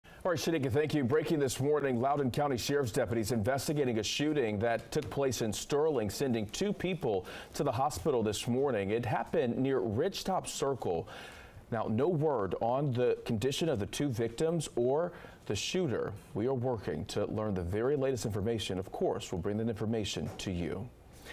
Two people taken to hospital after shooting in Sterling
The Loudoun County Sheriff’s Office said two people were taken to the hospital after a shooting in Sterling on Wednesday. The shooting happened in the 21000 block of Ridgetop Circle and prompted a heavy law enforcement presence in the area early Wednesday morning.